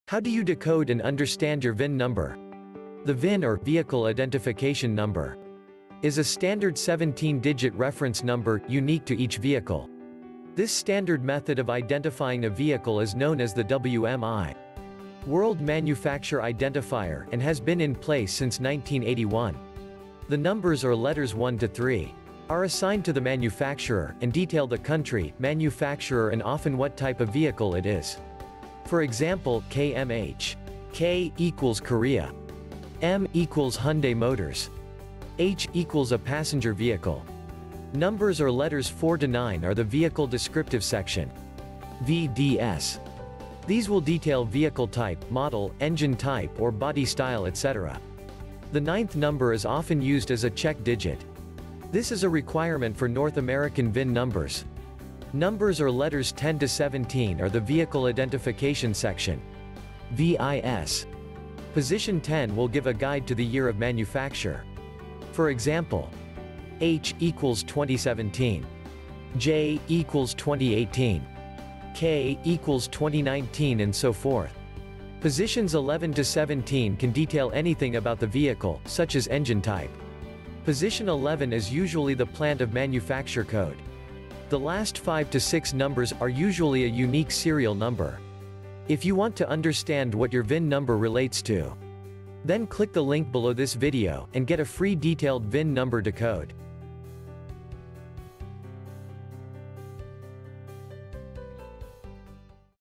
How To Decode Your VIN Number. VIN Number Decoder & Decoding
How To decode A VIN Number VIN Numbers Explained

VIN Numbers Explained Free VIN Number lookup and Decoding Guide. How to Lookup a vin number.
What is a VIN Number & How do I decode and understand my VIN.
The VIN vehicle identification number is a standard 17 digit reference unique to each vehicle. This standard method of identifying a vehicle is known as the WMI (World Manufacturer Identifier) and has been in place since 1981

Numbers/letters  1 to 3 (WMI)
These are assigned to the manufacturer (WMI) and will detail the company and where in the world they are based
Example:  KMH  K= Korea MH= Hyundai

Numbers/letters 4 to 9 are Vehicle Descriptive Section (VDS)
Will refer to a vehicles individual details including model, engine type, body style, seat belts,drive etc

The 9th number is often used as a check digit and is compulsory for cars made in North America
A Check Digit is calculated through a number of mathematical steps. Each letter used in a VIN has a corresponding number value and using the correct calculation the result can show if the VIN is correct.

Numbers/letters 10 to 17 are the Vehicle Identification Section (VIS)
Position 10
Will usually give a guide to the year of manufacture (not always the same as the year it was registered)
S = 1995 T = 1996 V = 1997 W = 1998 X = 1999
Y = 2000
1 = 2001 2 = 2002 3 = 2003 4 = 2004 5 = 2005
6 = 2006 7 = 2007 8 = 2008 9 = 2009

F = 2015 G = 2016 H = 2017  J = 2018 K = 2019
L = 2020  M = 2021 N = 2022 P = 2023 R = 2024

Positions 11
Is usually the Plant code

The rest to 17
Can detail anything on the vehicle although they are usually the production sequence numbers and therefore produce a unique serial

Video created with Video software.

आप कैसे समझ सकते हैं और अपने को समझें
विन संख्या। VIN या वाहन